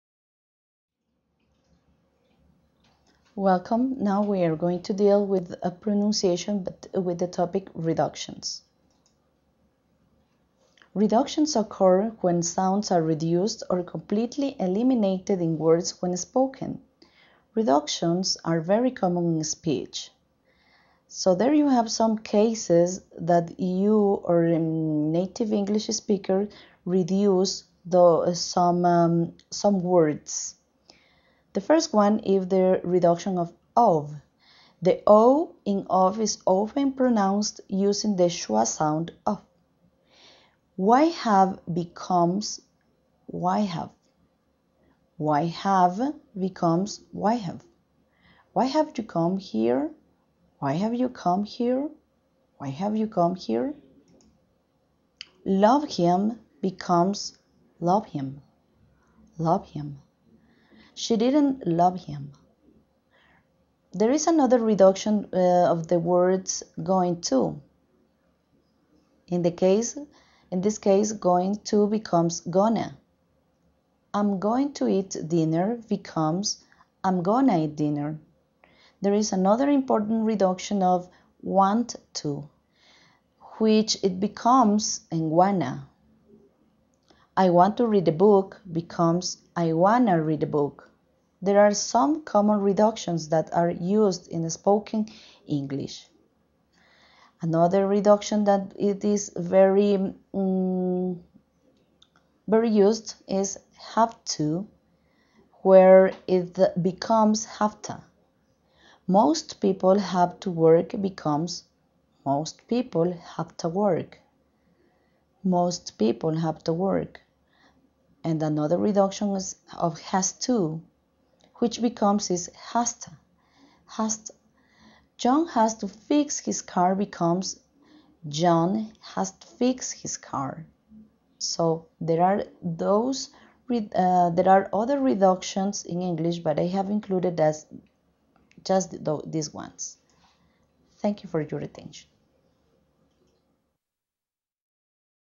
UTPL PRONUNCIATION: REDUCTIONS [(INGLÉS)(LISTENING AND SPEAKING II)]
Inglés
Listening and Speaking II
Tema: Pronunciation: Reductions
Ponente: Lic. Elsa Morocho